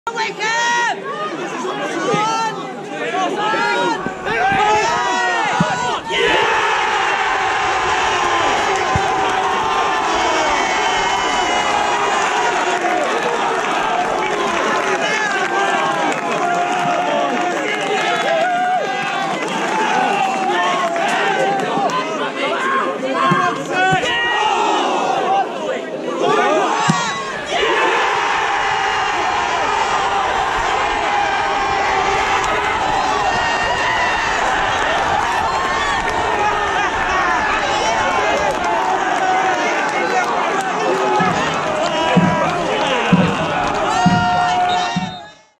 Fan Cam: Accrington 1-2 Wycombe
Watch how the heroic 190 Chairboys fans celebrated both Scott Kashket and Alex Samuel's goals at the Wham stadium, as Wanderers came from behind to snatch a late victory at the Wham Stadium.